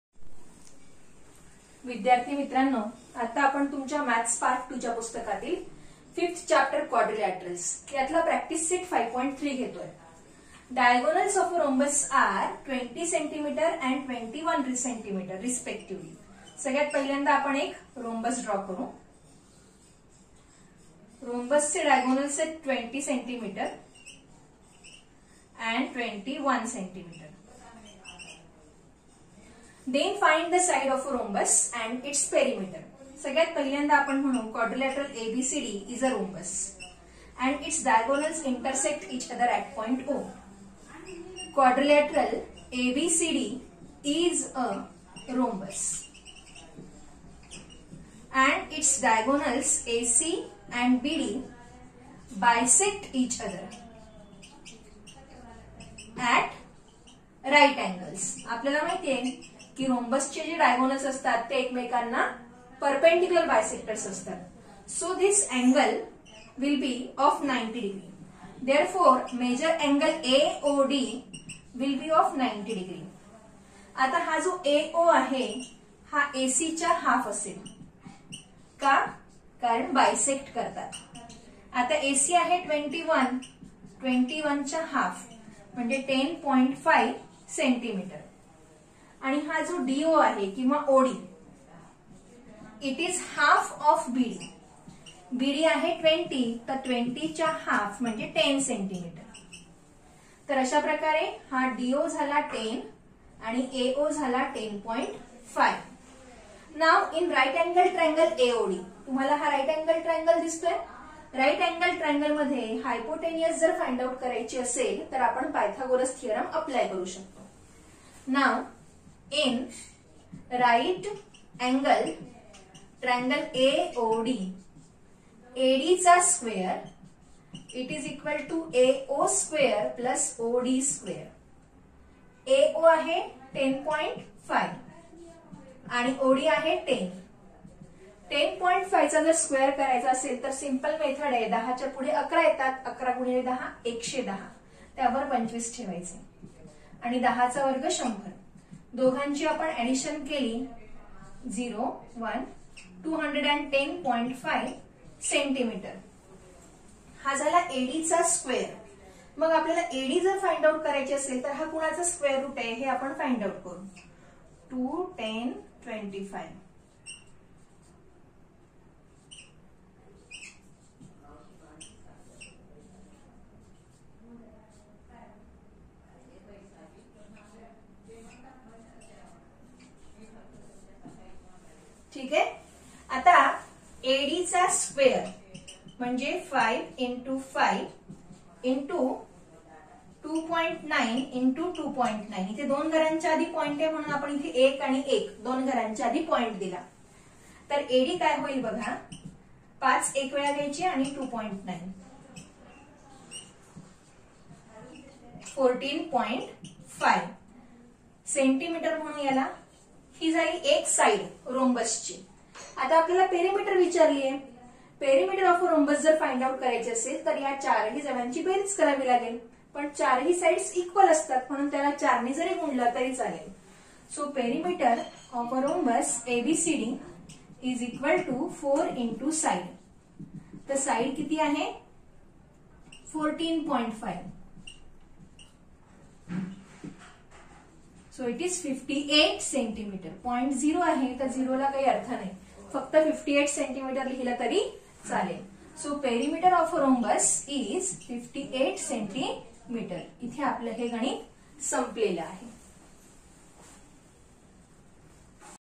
Diagonals of a rhombus are 20cm & 21cm respectively, then find the side of a rhombus and its peri.
9th standard, Mathematics Part 2, Chapter 5, Quadrilaterals, Practice set 5.3, Question No. 4 by Sharmila Deshmukh